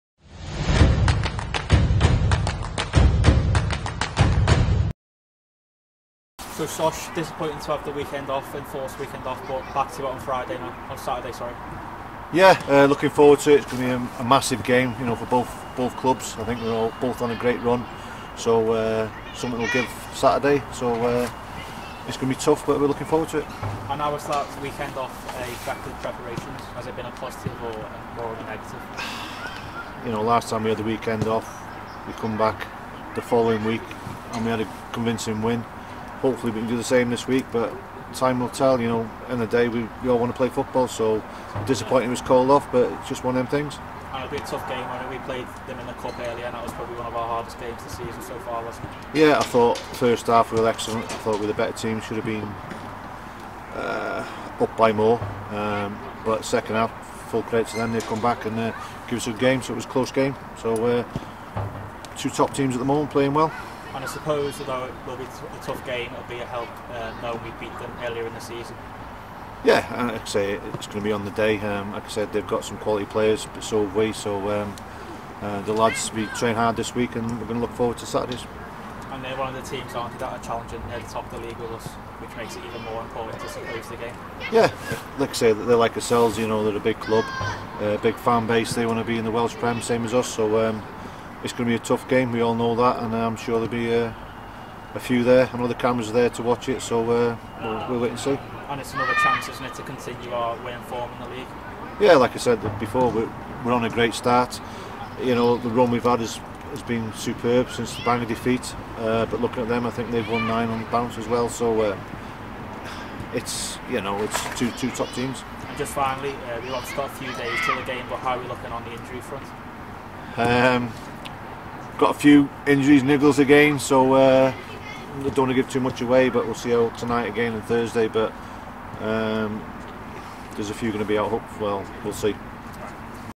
Steve O'Shaughnessy | Pre Match | Llandudno FC (JD Cymru North)
Wingmakers boss Steve O'Shaughnessy gives his thoughts ahead of this weekend's JD Cymru North top of the table clash against Llandudno FC.

By Joe Sargent